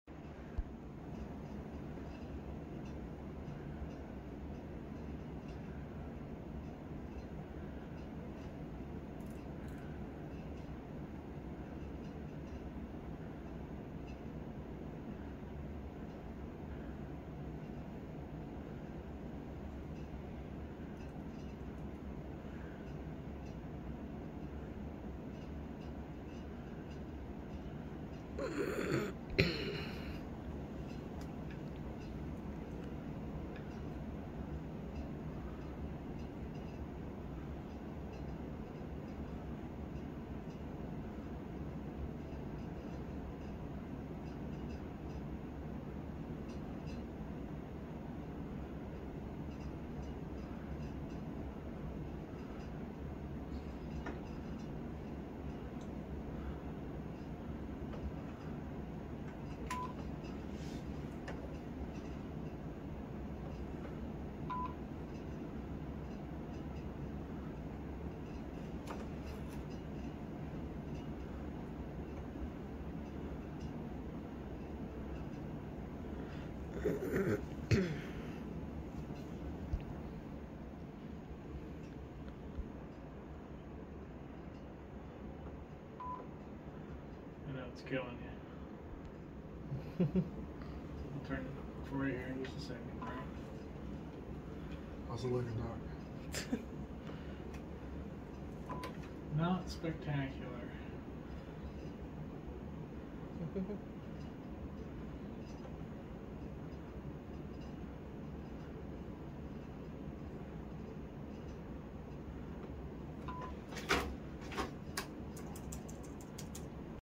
The Ultrasound of my Left Achilles Tear
This was cool to see, hurt like hell..lol